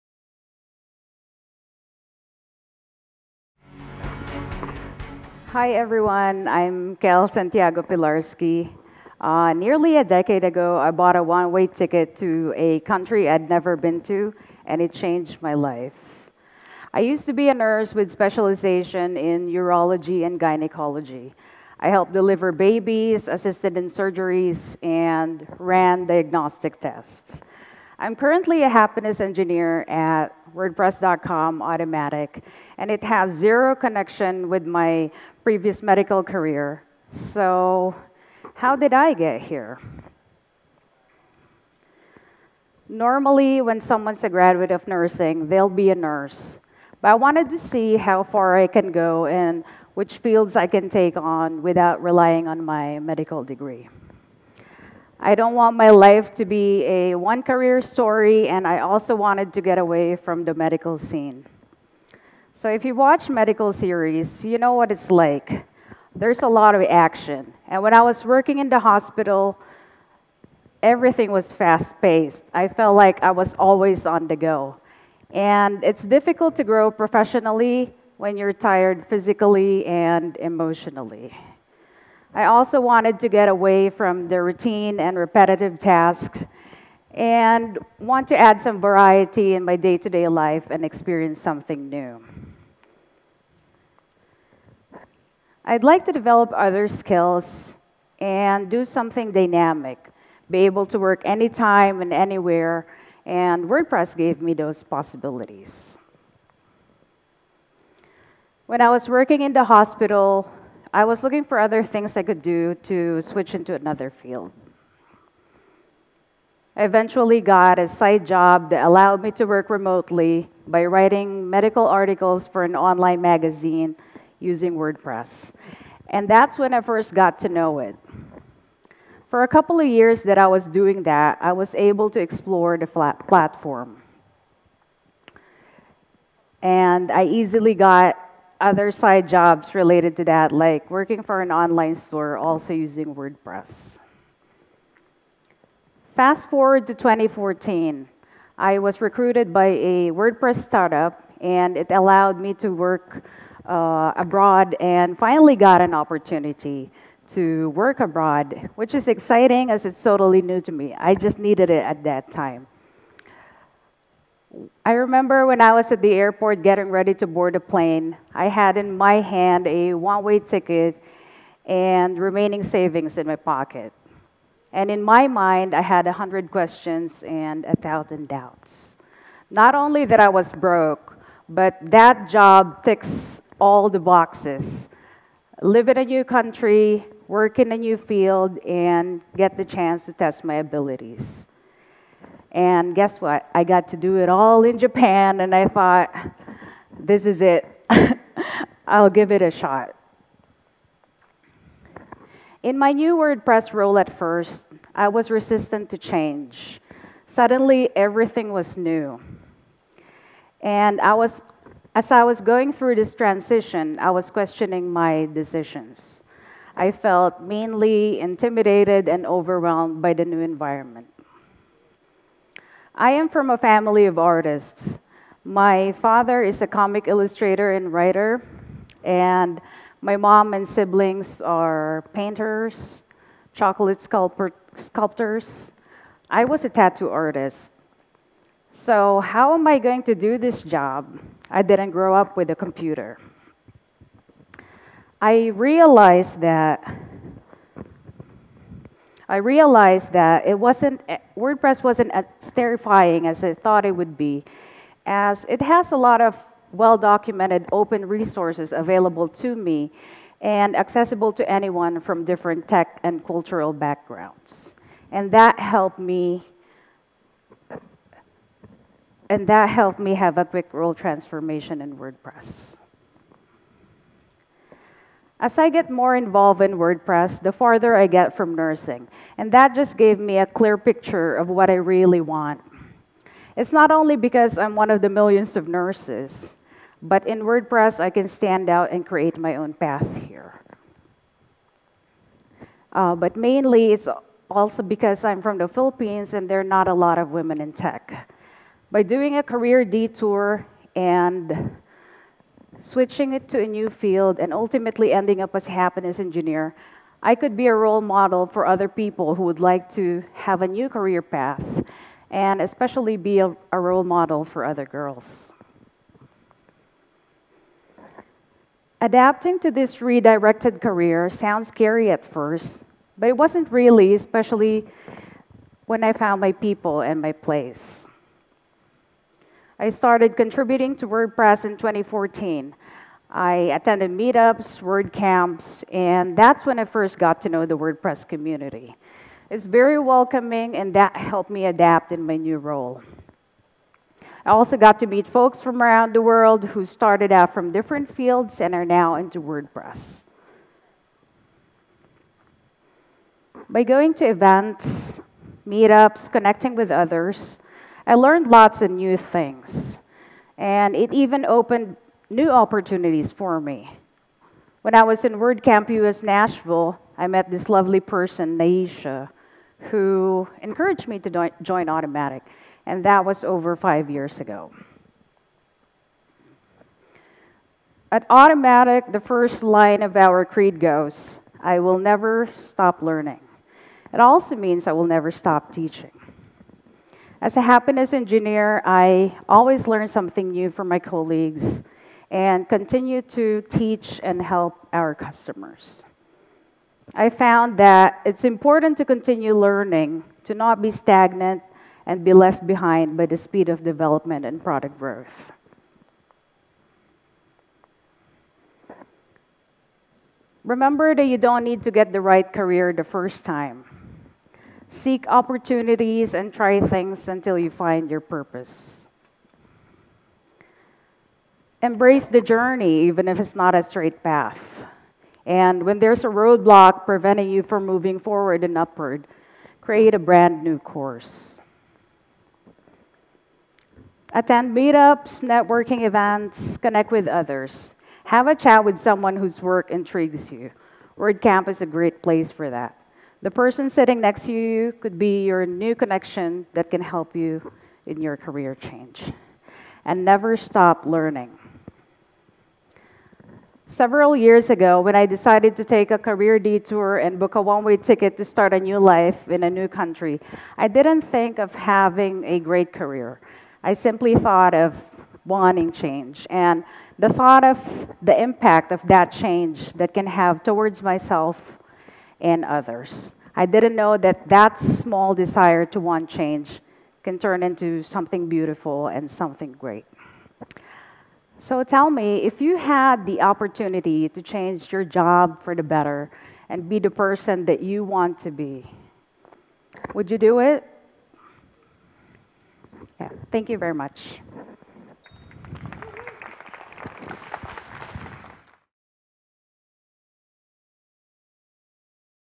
Career Detour: From the Medical Field to WordPress
This is a talk about the adventure and challenges of switching careers from two different fields: medical and WordPress. The route had a lot of unique challenges, colorful characters and transformative life scenarios.